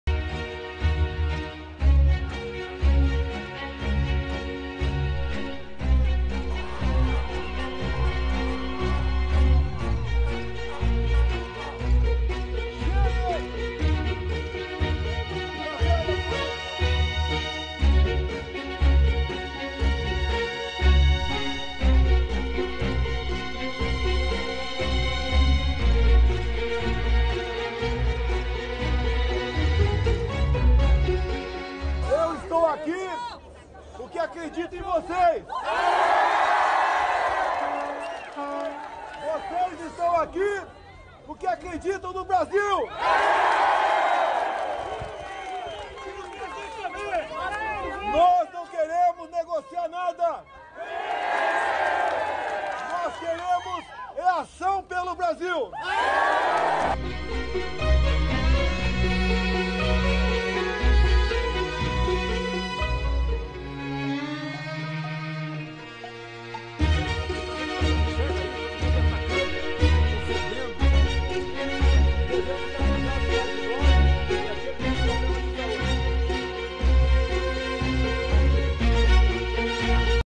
巴西总统参加反封锁集会：赞扬支持者们为“爱国者”
【巴西总统参加反封锁集会：不戴口罩且咳嗽】4月19日，巴西总统博索纳罗在首都出席支持者举办的集会并抨击疫情封锁令，还赞扬支持者们为“爱国者”。博索纳罗本人没有戴口罩，在演讲中不时咳嗽。博索纳罗的支持者还呼吁军队介入。巴西已是南美疫情最严重国家